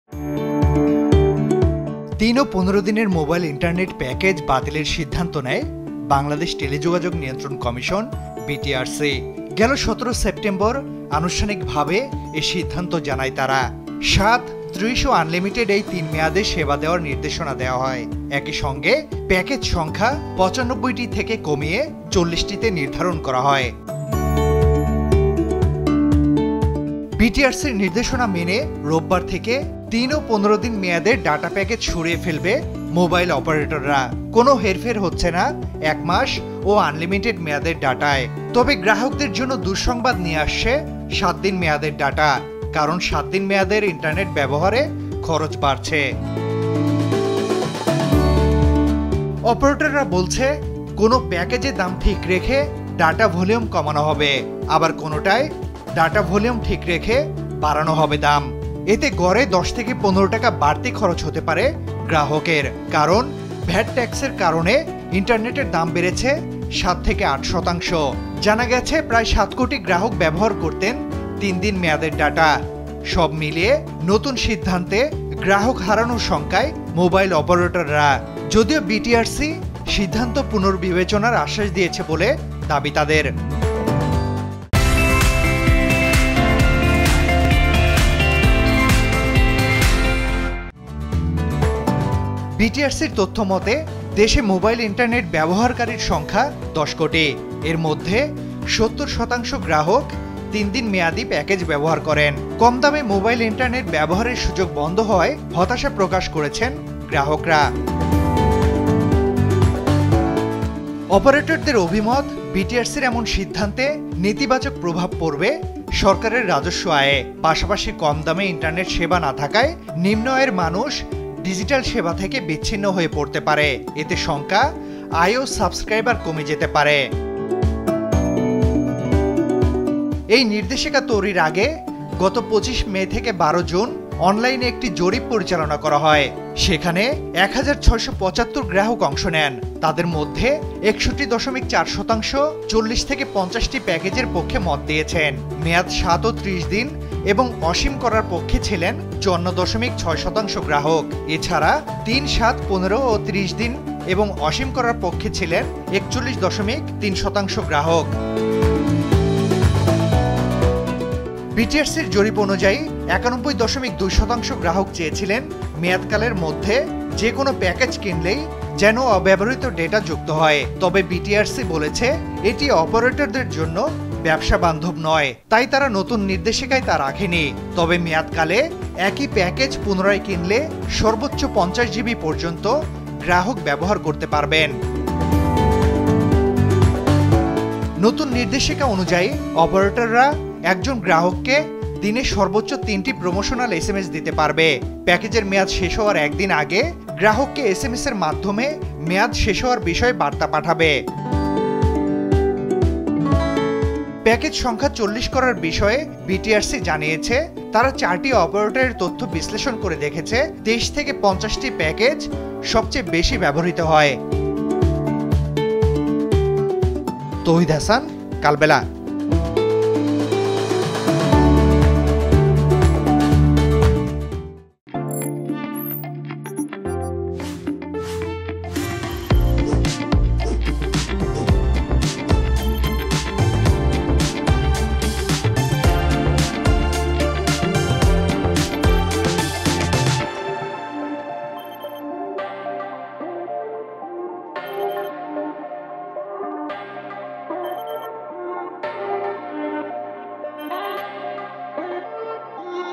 ইন্টারনেট ব্যবহারে খরচ বাড়ছে, স্বল্পমেয়াদি প্যাকেজ বাতিল | Internet Package Cost | Kalbela
৩ ও ১৫ দিনের মোবাইল ইন্টারনেট প্যাকেজ বাতিলের সিদ্ধান্ত নেয় বাংলাদেশ টেলিযোগাযোগ নিয়ন্ত্রণ কমিশন-বিটিআরসি। গেল ১৭ সেপ্টেম্বর আনুষ্ঠানিকভাবে এ সিদ্ধান্ত জানায় তারা। ৭, ৩০ ও আনলিমিটেড-এই তিন মেয়াদে সেবা দেয়ার নির্দেশনা দেওয়া হয়। একইসঙ্গে প্যাকেজ সংখ্যা ৯৫টি থেকে কমিয়ে ৪০টিতে নির্ধারণ করা হয়। ইন্টারনেট ব্যবহারে খরচ বাড়ছে, স্বল্পমেয়াদি প্যাকেজ বাতিল | Internet Package Cost | Kalbela